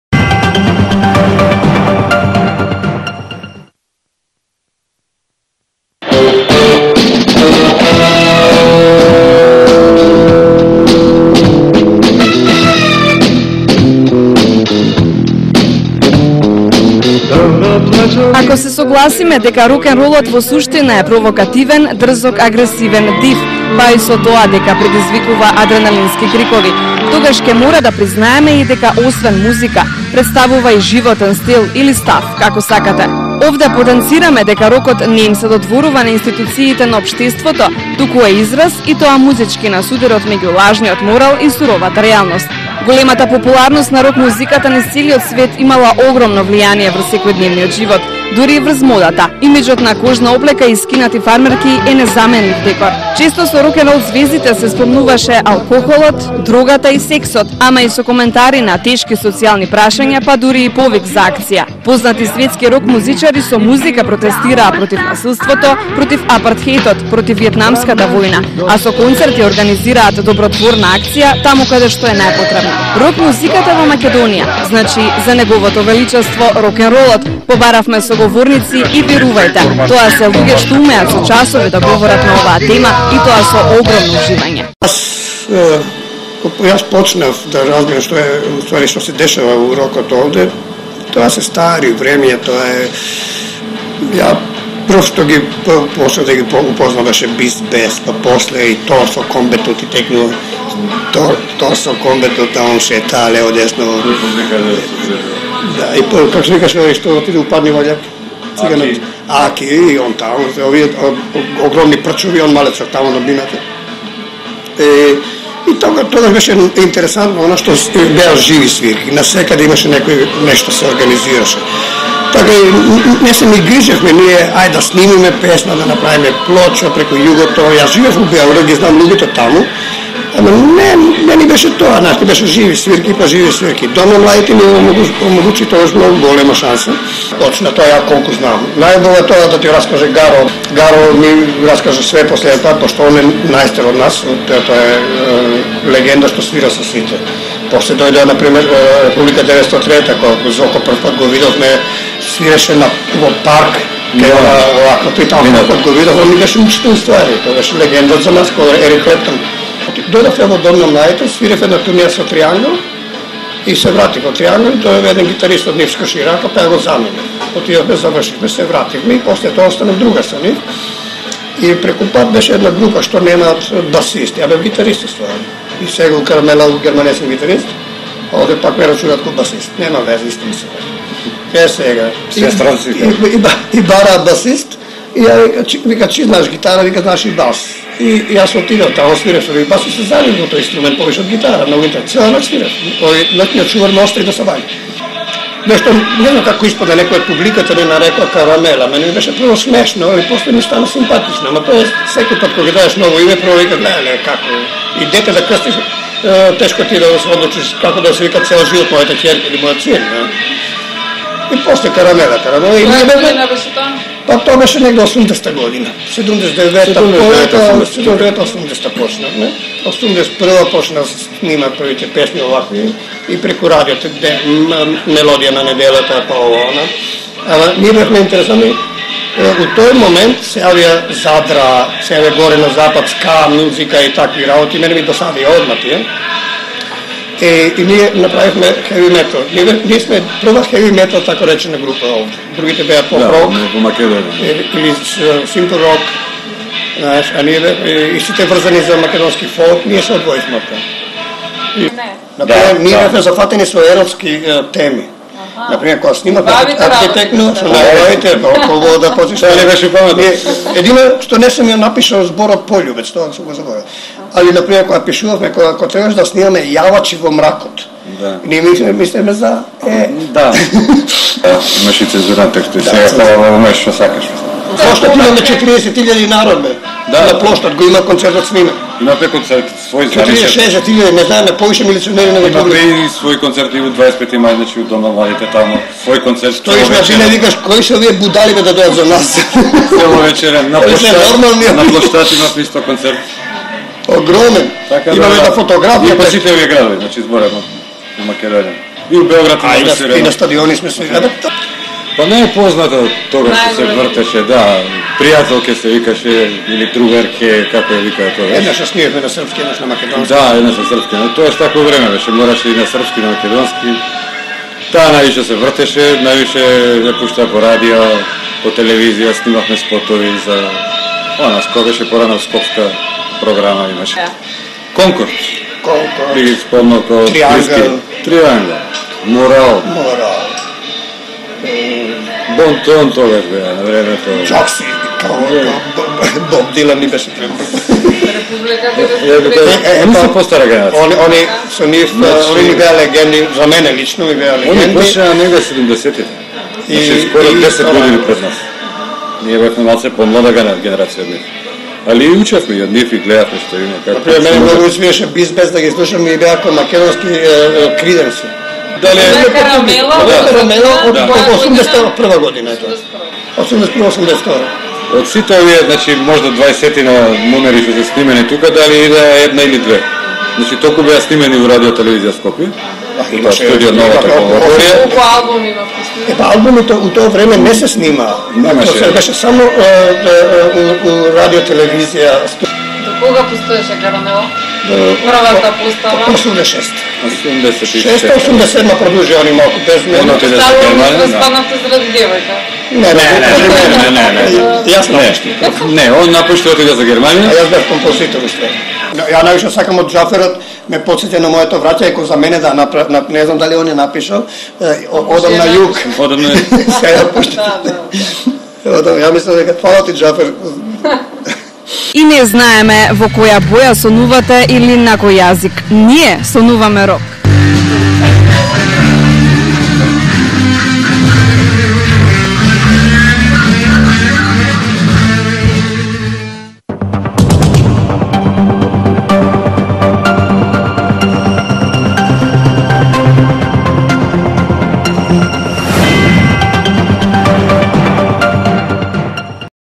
Interview KARAMELA 2011_PART 1.avi
Intervju za TV kanal 24 na tema: Rok muzika vo Makedonija